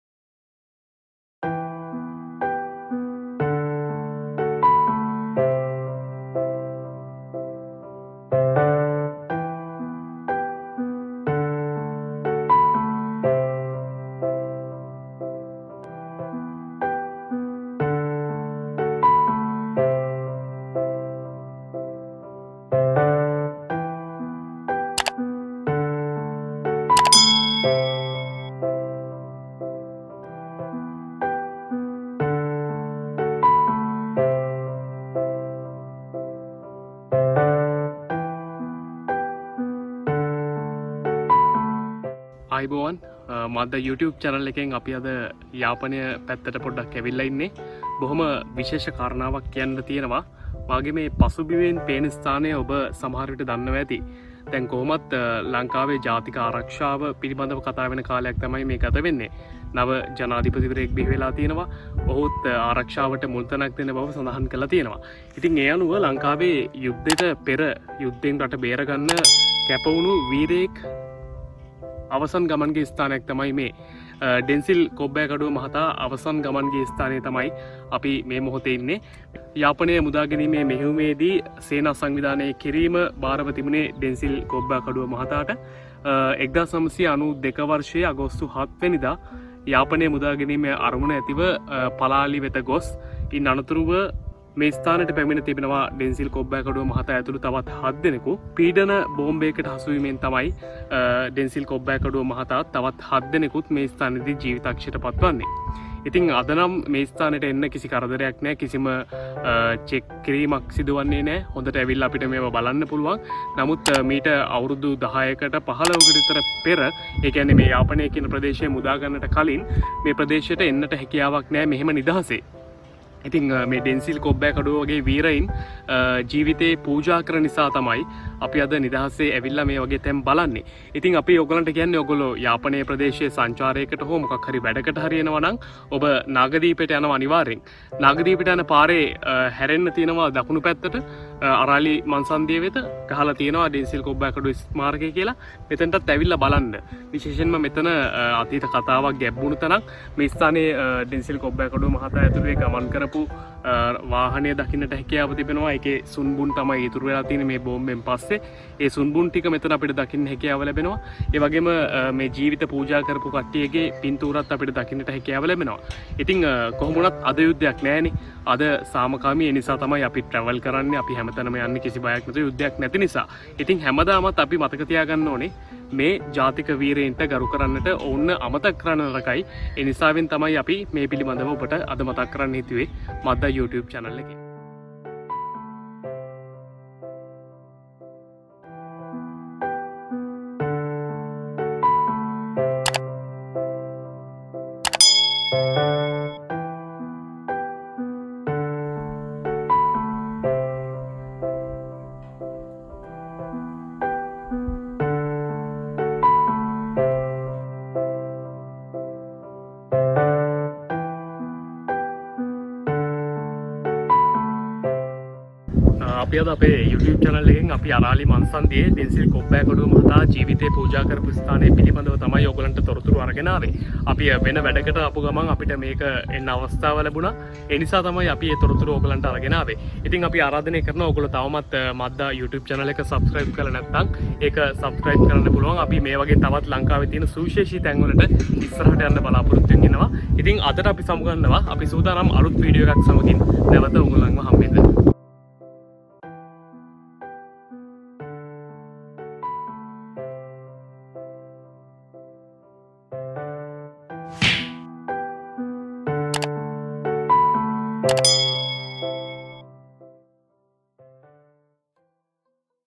Densil Kobbakaduwa Memorable Place | Madda Travel | Vlog #07
Unsung memories of a national hero.
Densil Kobbakaduwa is a true hero. These heroes are the first owners of the military victory we have achieved today as a country. Even though they are not alive today, we are forever indebted to them for every breathe we take.